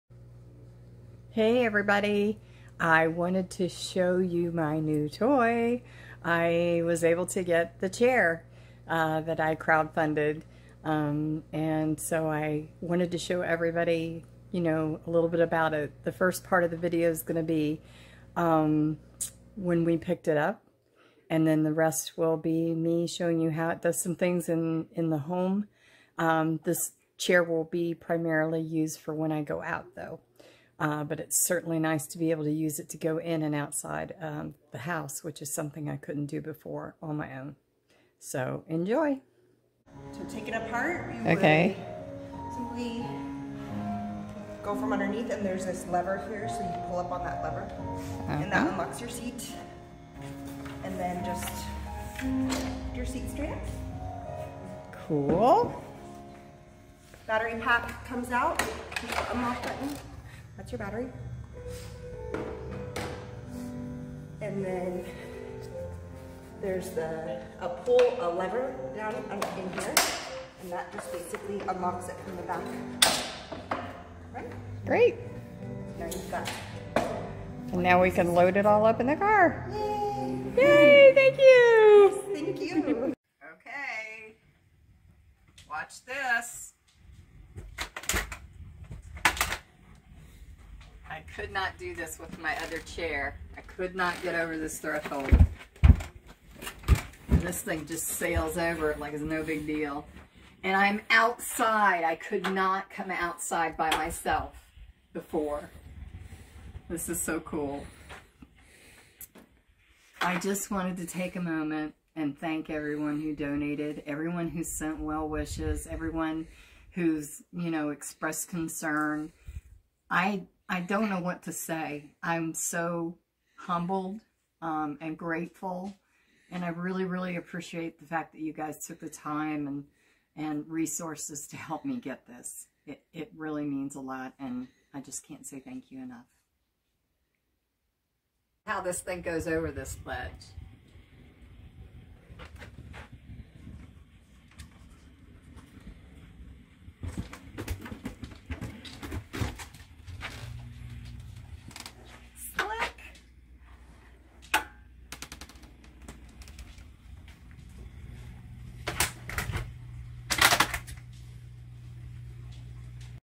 My new Whill Ci2 power chair!
I wanted to show everyone my new Whill Ci2 power chair. For me, this chair will be used primarily as a portable option when out in public, as I'm finding it increasingly difficult to use my manual chair after double mastectomy surgery. Thanks to all who helped make this possible! A special thanks to Help Hope Live for their crowdfunding platform and support, Alaska Mobility LLC in Anchorage, Alaska for working with me to make this possible, and everyone who helped with my crowdfunding campaign including those who donated, shared, and showed support. Thanks everyone!